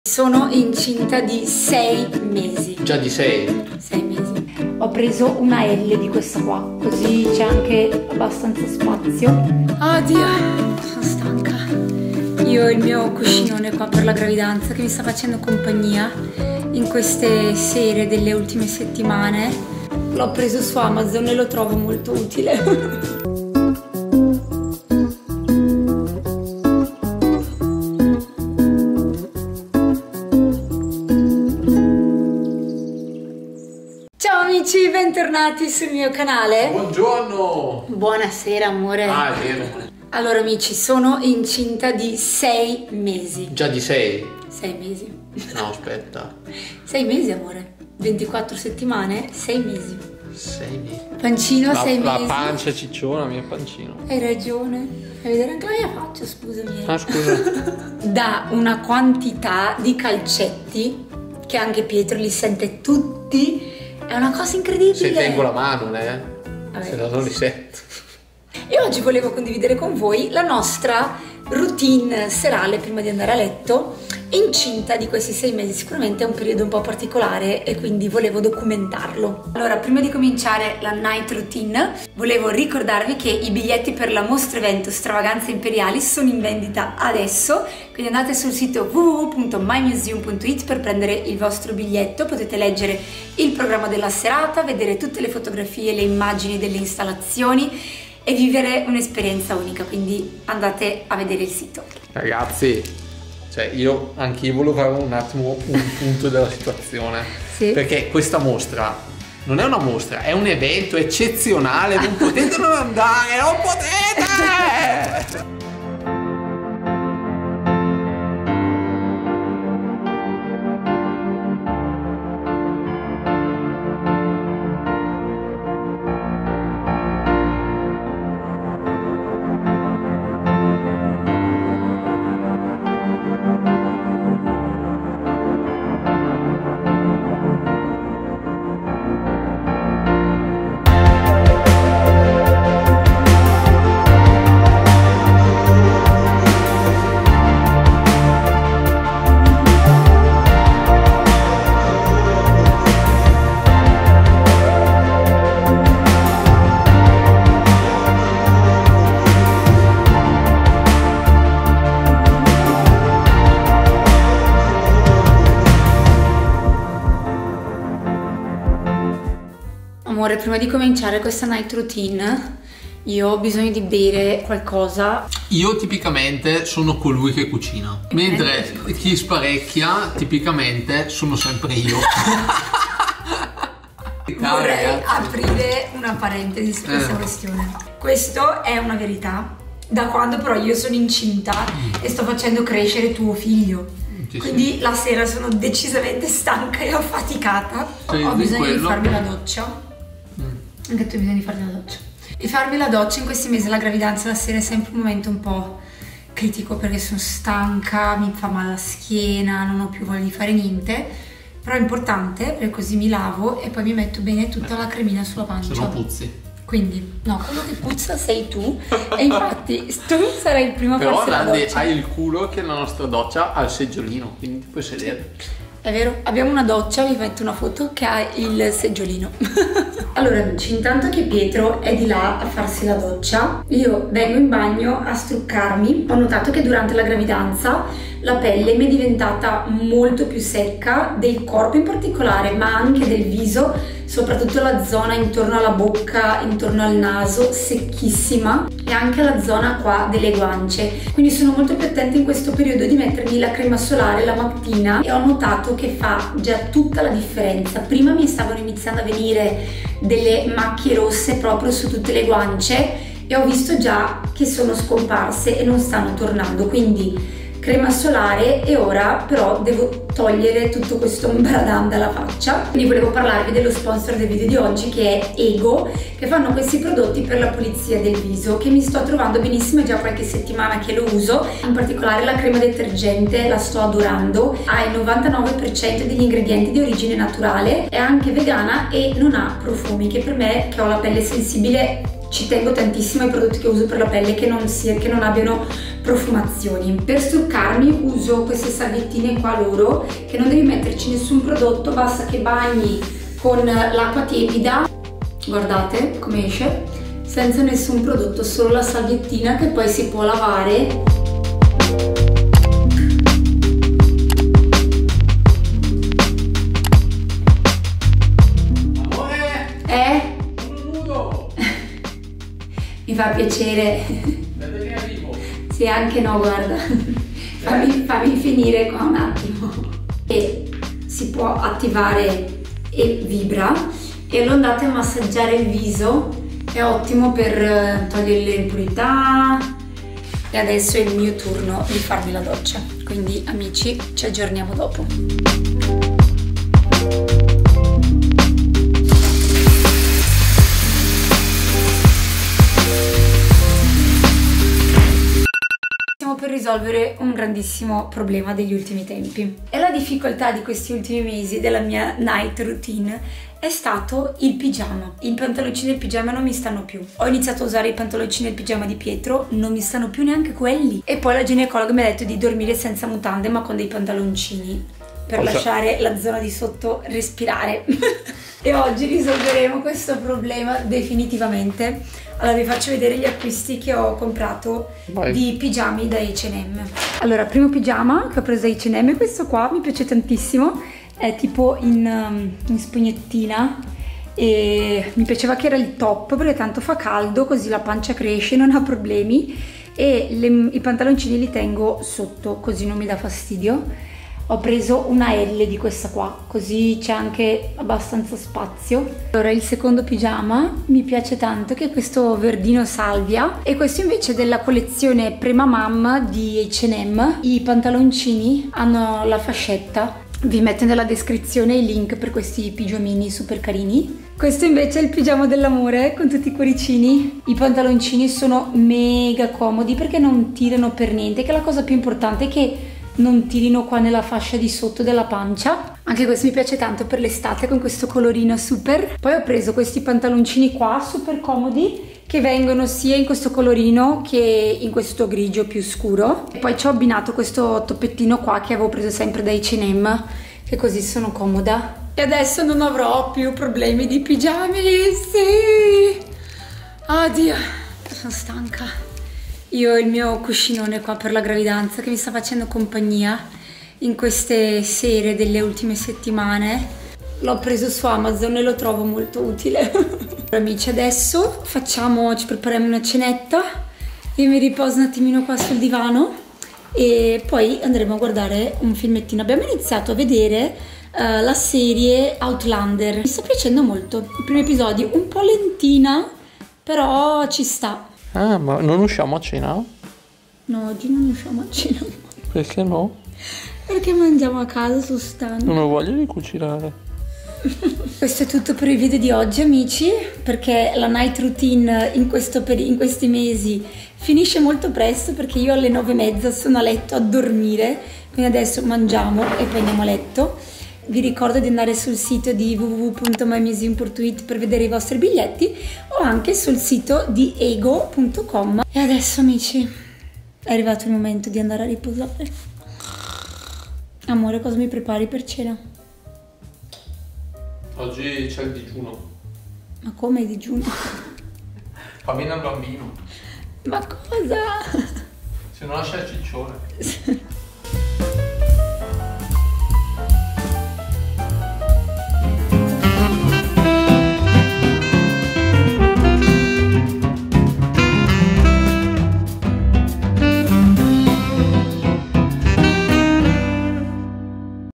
La mia ROUTINE SERALE in GRAVIDANZA - NIGHT ROUTINE 🌙🤰+ EGGO review!
La mia night routine incinta di 6 mesi! Diciamo solo che vado a letto moooolto prima... 😁

✨10% di sconto con codice NANDI10 per tutti i prodotti di EGGO